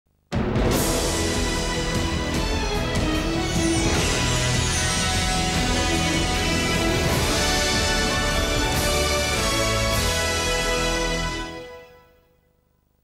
Nine - Sydney ident (October 1998)
From a recording of the 1998 Alfred Dunhill Cup on TCN-9 Sydney on 12 October 1998.